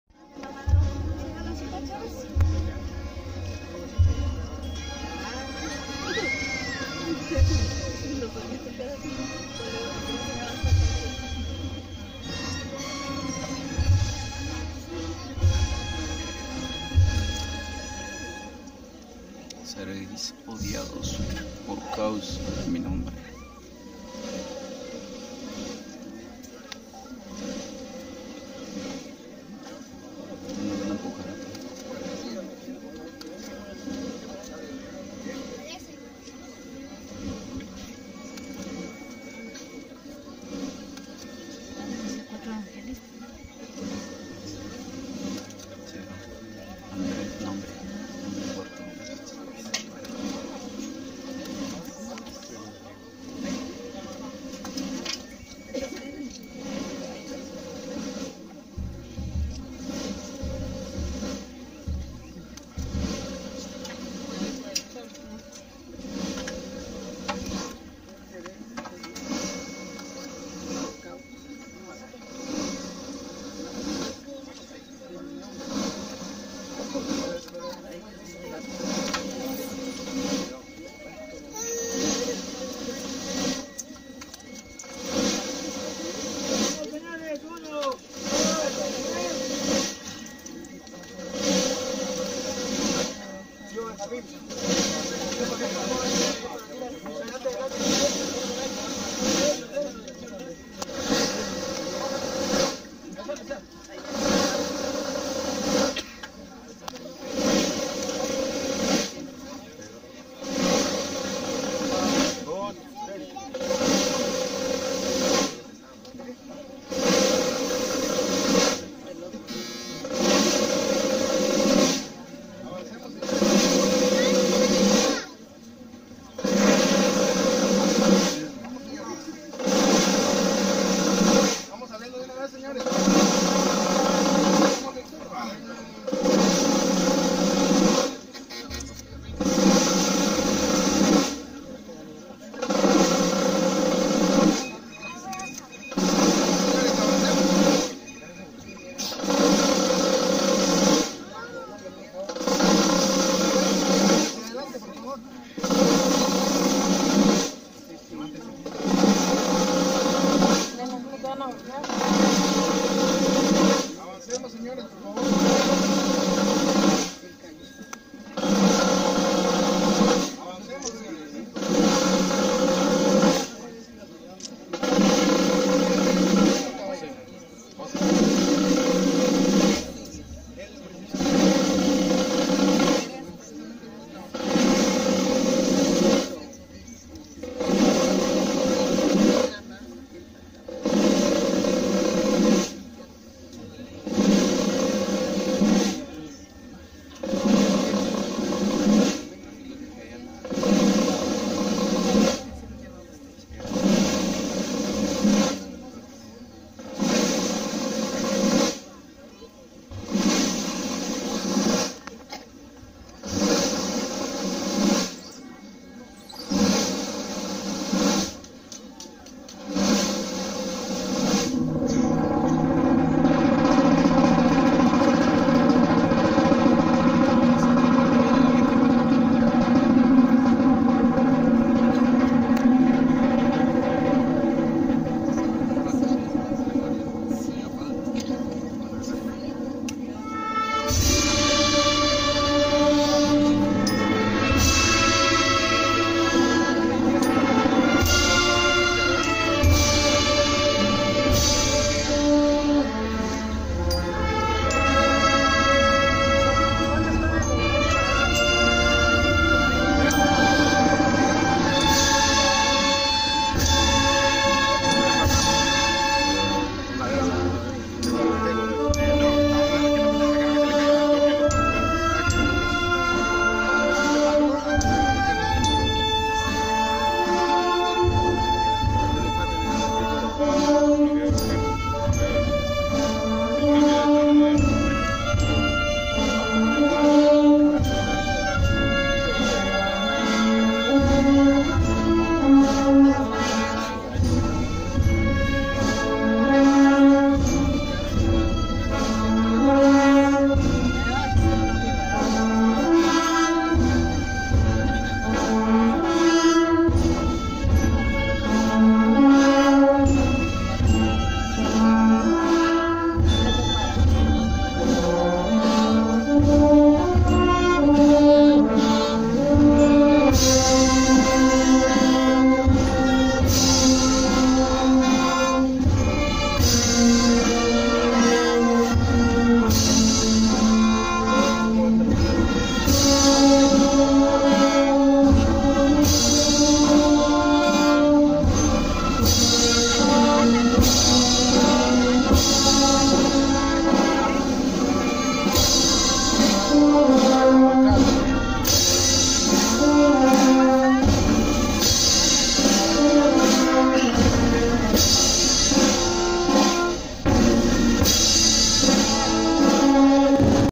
Jesús de la Caída 2019
Procesión de la consagrada Imagen de Jesús Nazareno de la Caída, San Bartolomé Becerra.